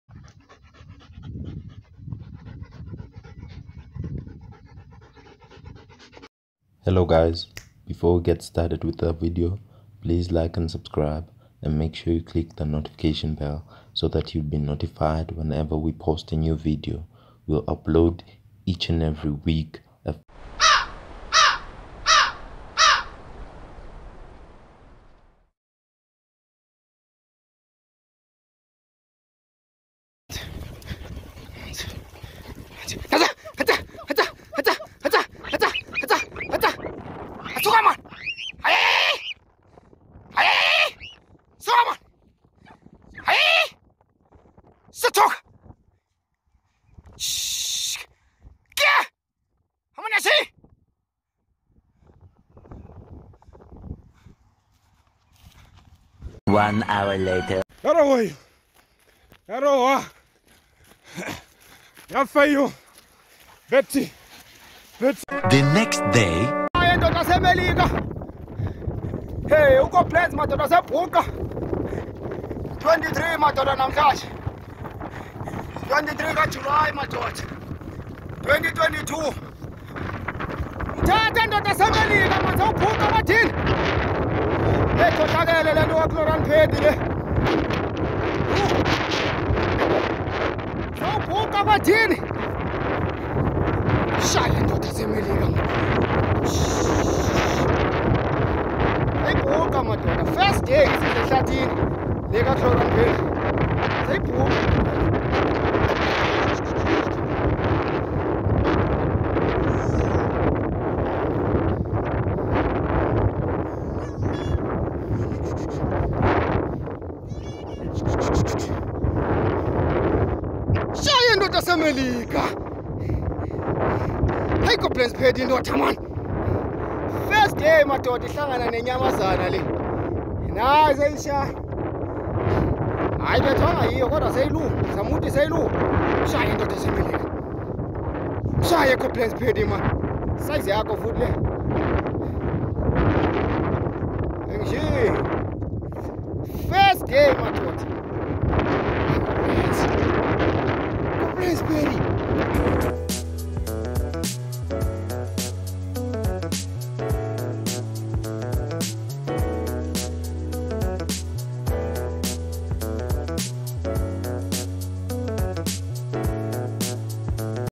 GREYHOUND DOGS VS STEENBOK 2022 p1
Amaphisi hunters is a hunting channel in africa using sighthounds. Hopefully you guys enjoy our content.Thanks for tuning in😄